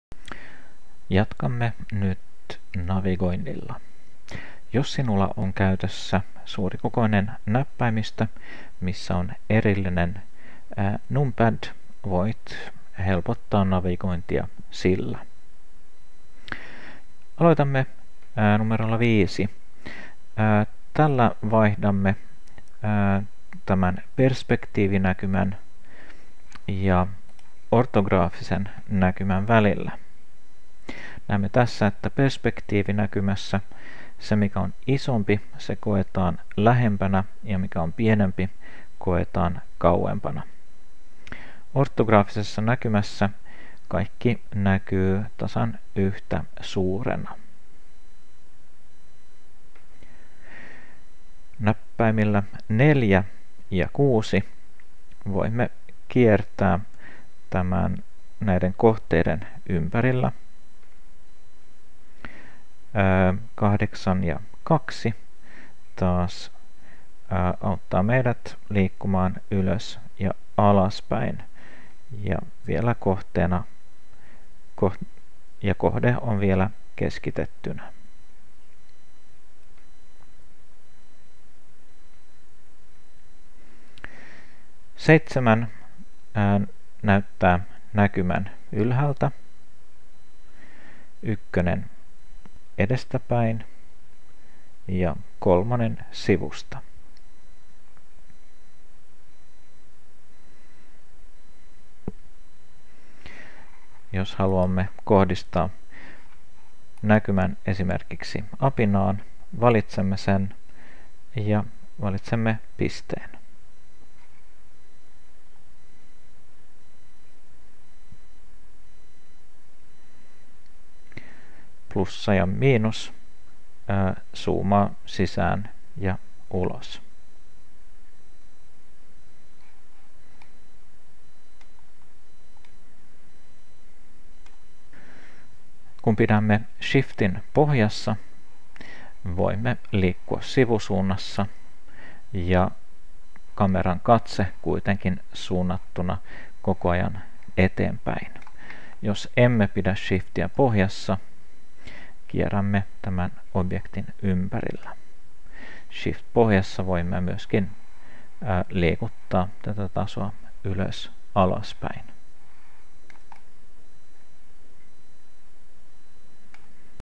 Luukku 3
Navigointi näppäimistöllä, jos sinulla ei ole täyskokoista, voit läppärissä painaa NumLock ja kuitenkin käyttää numeronäppäimiä.